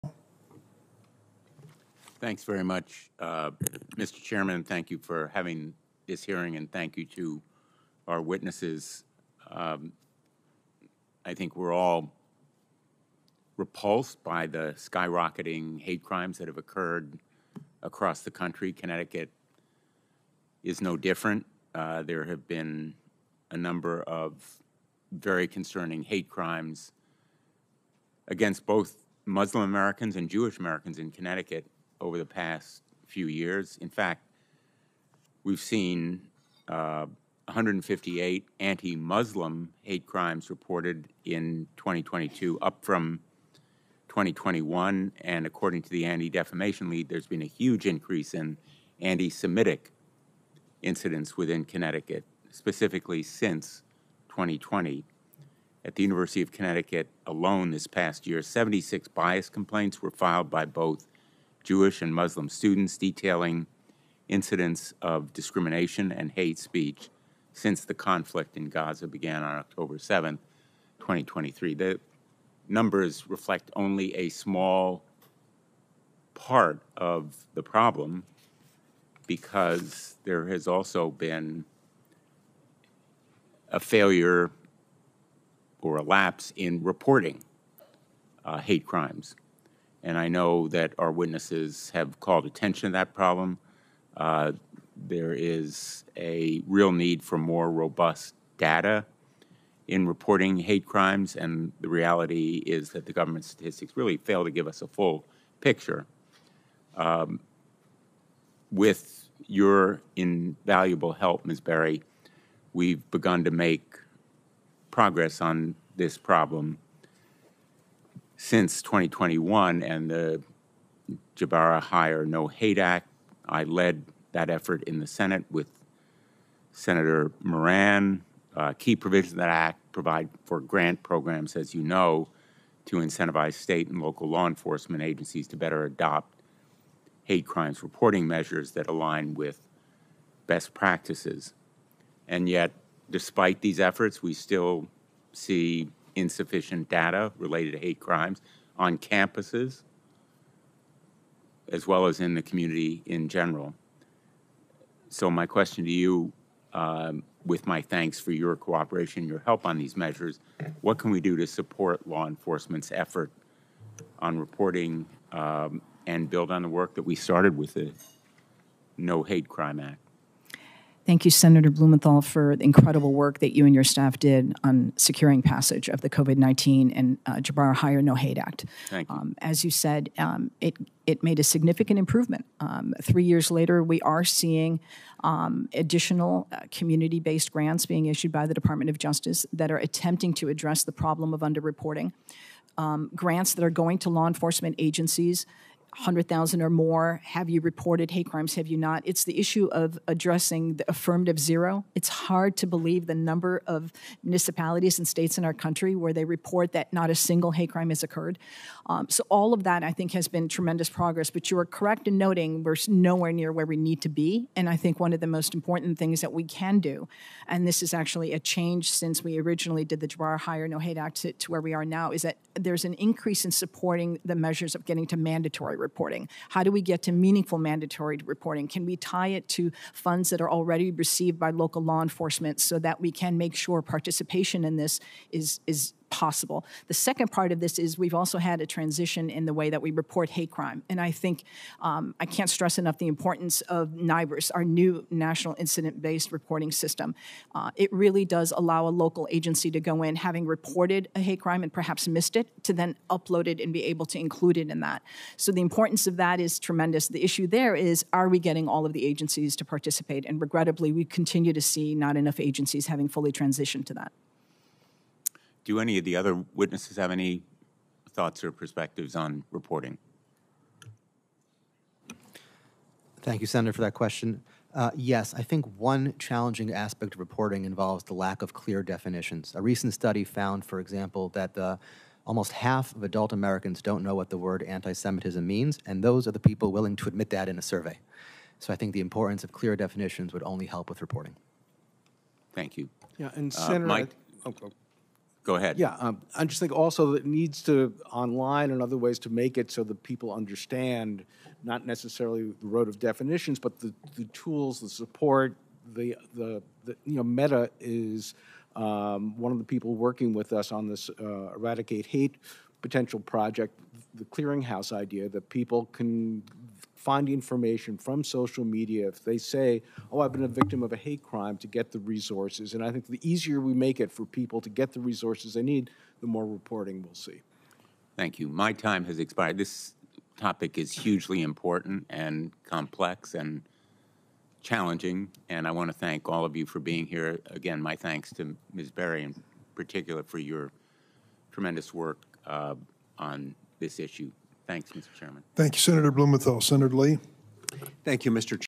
Richard Blumenthal Asks Experts About Addressing Challenges Related To Reporting Hate Crimes
During a Senate Judiciary Committee hearing prior to the Congressional recess, Sen. Richard Blumenthal (D-CT) questioned witnesses about hate crimes and the challenges that arise with reporting.